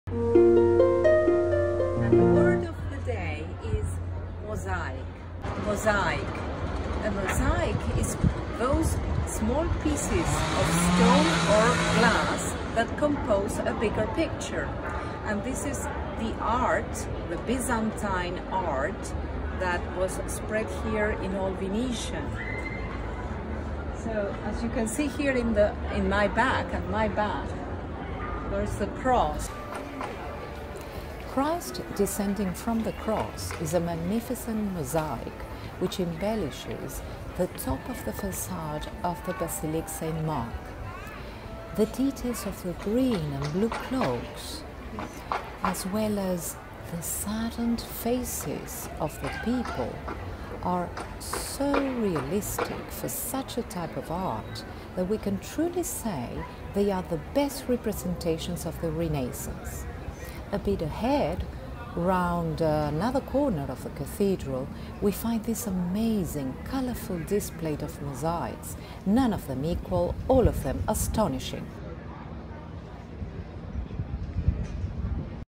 MOZAIC (EN)_¿Qué significa? | Inglés Nivel Avanzado B2-C1
😎 Hoy paseamos por Venecia y fuimos hasta la más icónica Basílica para hablarte de una de las forma de Arte Pictórico más importante en la ciudad de los Doges. ¿Te animas a descubrirla?

✍ TELL US: Where else have you seen this art? In your town/country/region? How did you find out?

🎤 ¿Sabes cómo PRONUNCIAR éstas palabras correctamente?
- MOZAIC
- BYZANTAINE Art
- RENAISSANCE
Pues qué esperas? Dale CLICK!

🔔 Comparte y dale a la campanita para enterarte de todos los nuevos videos.
💖 You can follow my classes online: @elisendana
💬 Contact via msg, Instagram or FB